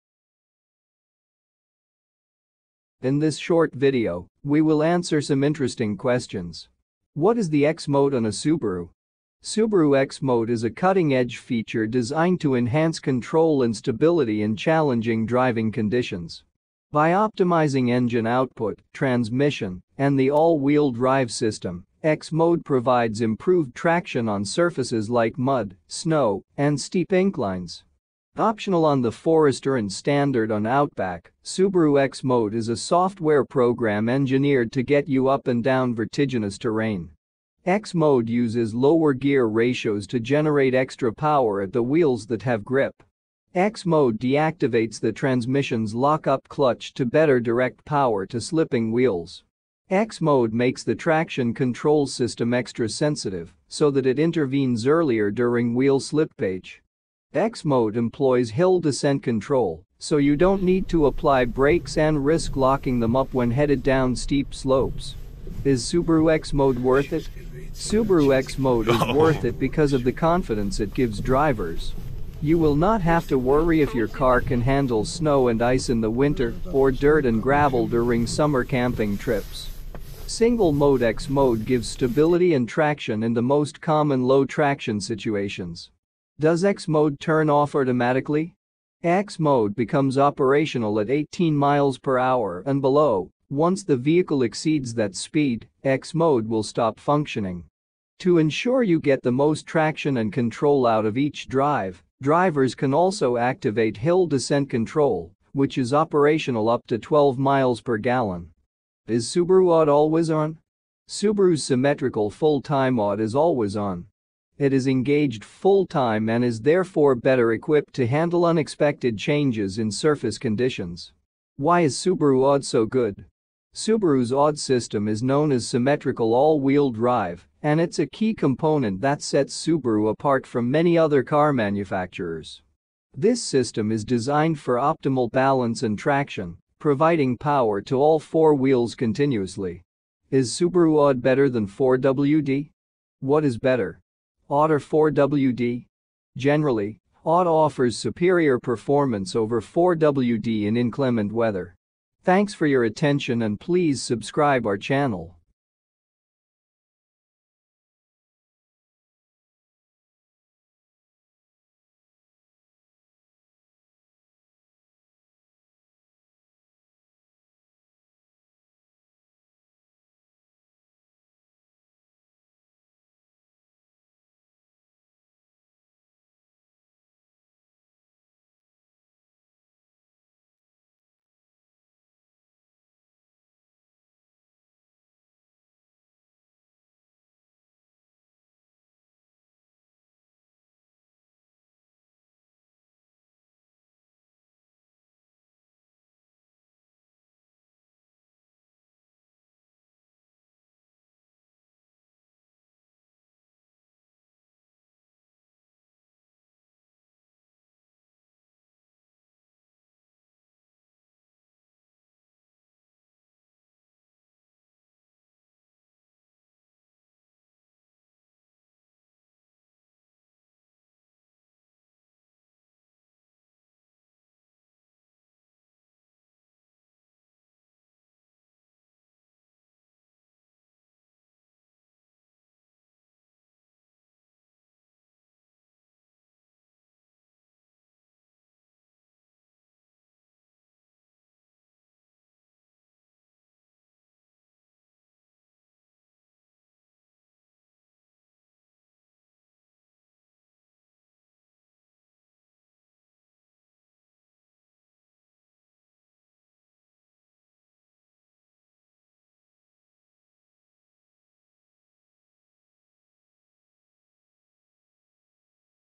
Subaru AWD with X-MODE vs Sheet of snow! What is Subaru X-Mode and how does it work?
Subaru AWD with X-MODE vs Sheet of snow! 2020 Subaru Forester 2.5i Limited. What is Subaru X-Mode and how does it work?